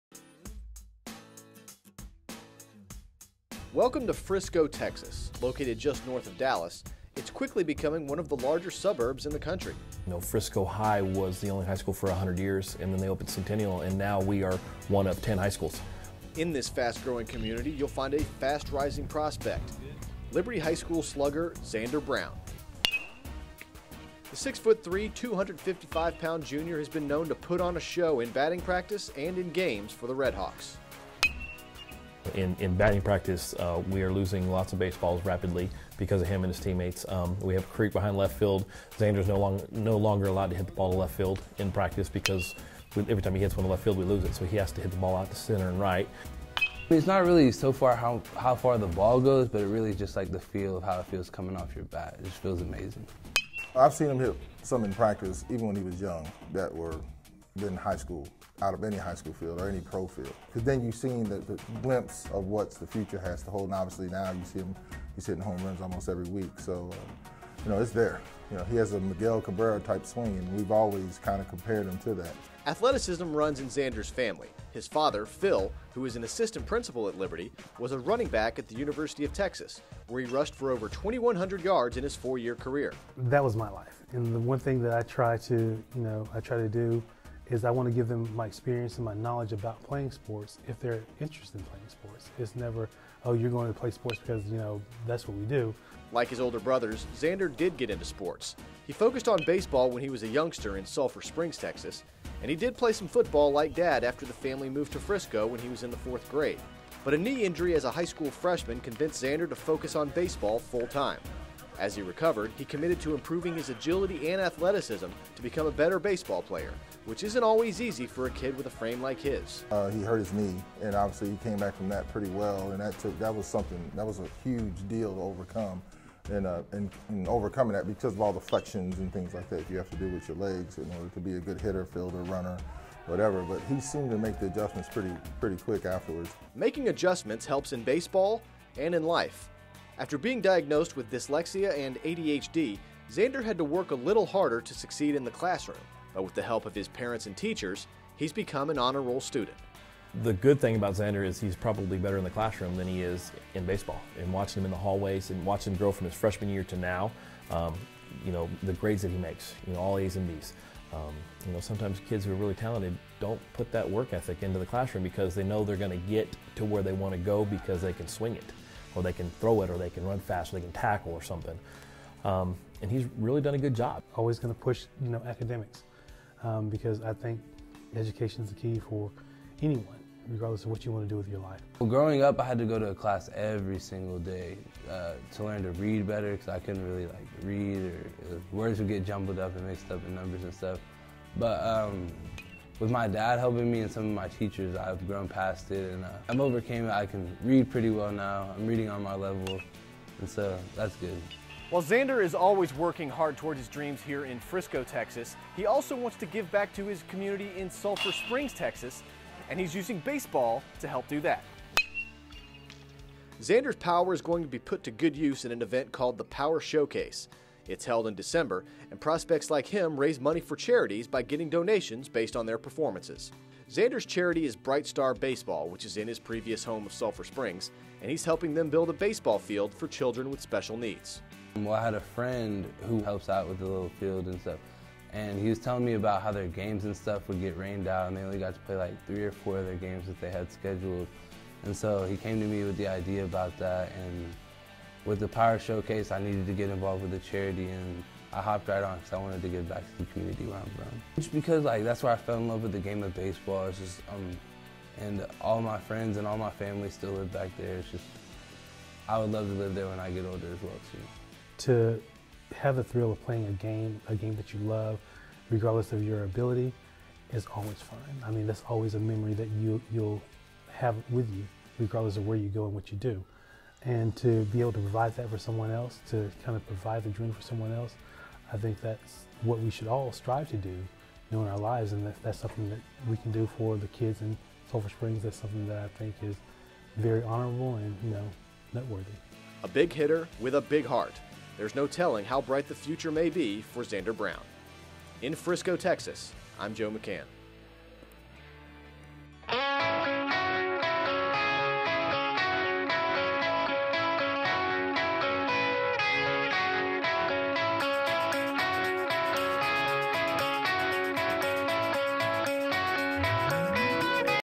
Zander Brown - Frisco Liberty Baseball - Interview - Sports Stars of Tomorrow
Zander Brown is a power-hitting first baseman at Frisco Liberty High School in Texas. His dad, Phil, is an assistant principal at the school and played football for the Texas Longhorns. Zander is using baseball to help special needs children in his hometown of Sulphur Springs, TX.

Some video courtesy the Power Showcase.